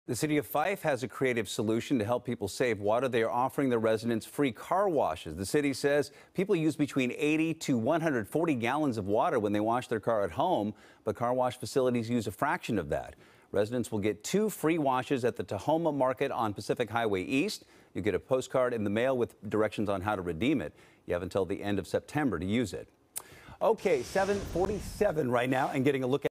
City of Fife gives residents free car washes | FOX 13 Seattle
The city of Fife is offering residents free car washes in an effort to help people save water.